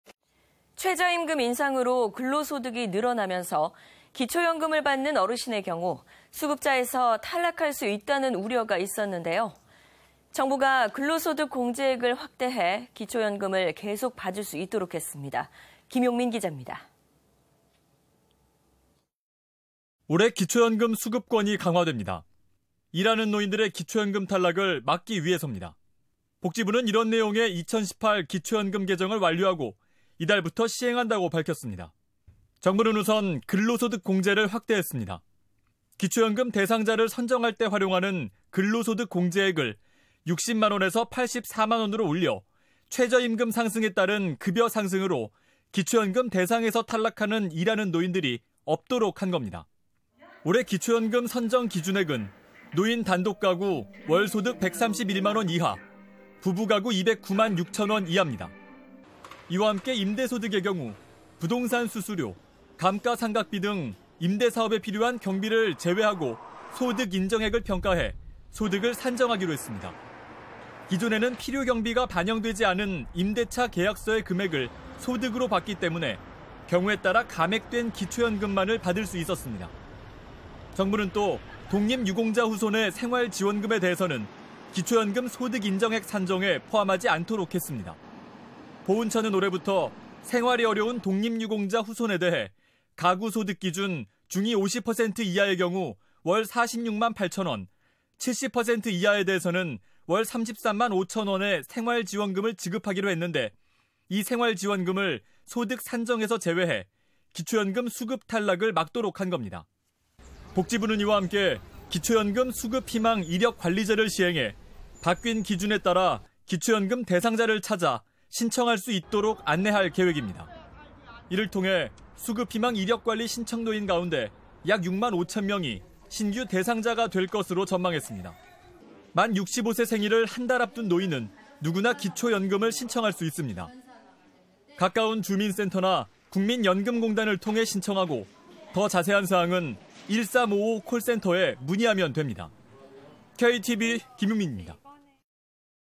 '일하는 노인' 기초연금 탈락 방지…공제액 상향
최저임금 인상으로 근로소득이 늘어나면서, 기초연금을 받는 어르신의 경우 수급자에서 탈락할 수 있다는 우려가 있었는데요.
정부가 근로소득공제액을 확대해 기초연금을 계속 받을 수 있도록 했습니다.
김용민 기자의 보도입니다.

올해부터 기초연금 수급권이 강화됩니다.
일하는 노인들의 기초연금 탈락을 막기 위해서입니다.
복지부는 이런 내용의 2018 기초연금 개정을 완료하고, 이달부터 시행한다고 밝혔습니다.
정부는 우선 근로소득 공제를 확대했습니다.
기초연금 대상자를 선정할 때 활용하는 근로소득 공제액을 60만 원에서 84만 원으로 올려, 최저임금 상승에 따른 급여 상승으로 기초연금 대상에서 탈락하는 일하는 노인들이 없도록 한겁니다.
올해 기초연금 선정기준액은 노인 단독가구 월 소득 131만 원 이하, 부부가구 209만 6천 원 이하입니다.
이와 함께 임대 소득의 경우 부동산 수수료, 감가상각비 등 임대사업에 필요한 경비를 제외하고, 소득인정액을 평가해 소득을 산정하기로 했습니다.
기존에는 필요경비가 반영되지 않은 임대차 계약서의 금액을 소득으로 봤기 때문에 경우에 따라 감액된 기초연금만을 받을 수 있었습니다.
정부는 또 독립유공자 후손의 생활지원금에 대해서는 기초연금 소득인정액 산정에 포함하지 않도록 했습니다.
보훈처는 올해부터 생활이 어려운 독립유공자 후손에 대해 가구소득 기준 중위 50% 이하의 경우 월46만 8천 원, 70% 이하에 대해서는 월 33만 5천 원의 생활지원금을 지급하기로 했는데, 이 생활지원금을 소득산정에서 제외해 기초연금 수급 탈락을 막도록 한겁니다.
복지부는 이와 함께 기초연금 수급희망 이력관리제를 시행해, 바뀐 기준에 따라 기초연금 대상자를 찾아 신청할 수 있도록 안내할 계획입니다.
이를 통해 수급희망 이력관리 신청 노인, 가운데 약 6만 5천명이 신규 대상자가 될 것으로 전망했습니다.
만65세 생일을 한 달 앞둔 노인은 누구나 기초 연금을 신청할 수 있습니다.
가까운 주민센터나 국민연금공단을 통해 신청하고 더 자세한 사항은 1355 콜센터에 문의하면 됩니다.
KTV 김용민입니다.